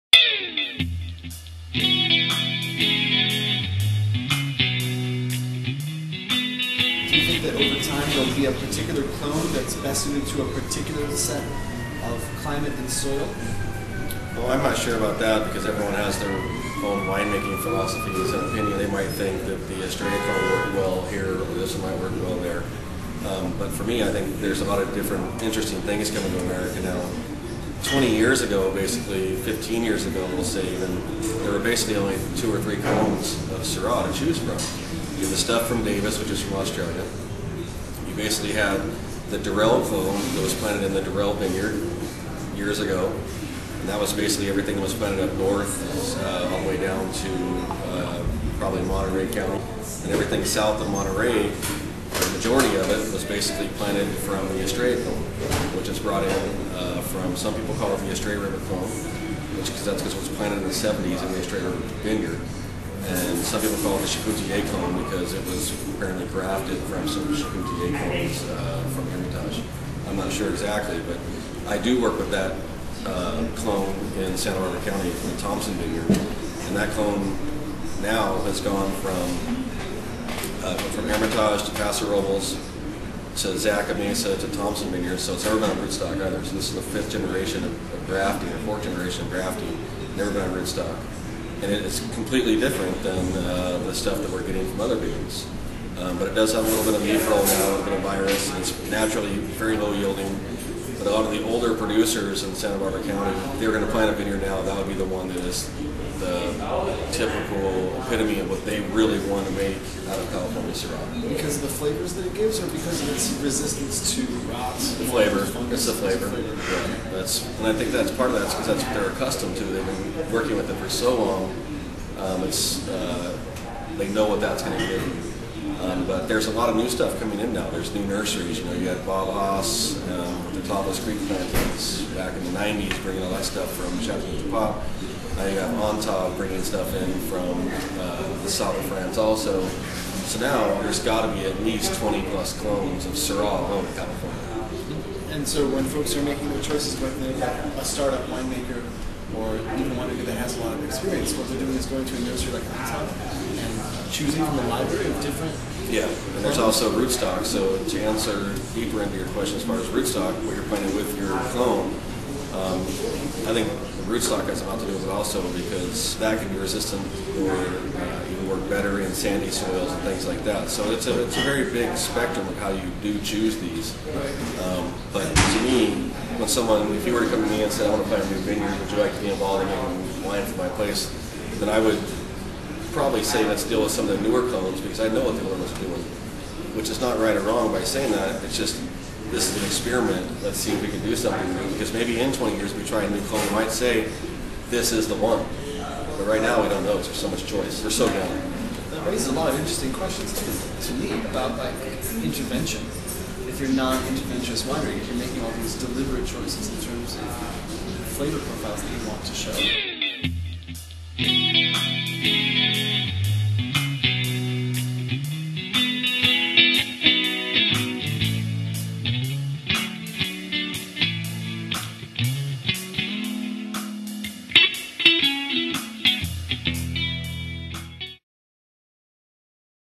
Tensley on the selection and history of Syrah clones in Santa Barbara, CA - Part 1 of 3
Winemaker Joey Tensley responds to Sommelier Jeremy Quinn's questions about the history of Syrah clones in Santa Barbara, California.  This is the first of three videos exploring clones, rootstock, grafting and the planting of Syrah in Santa Barbara.

Ask a Winemaker on Twitter @askawinemaker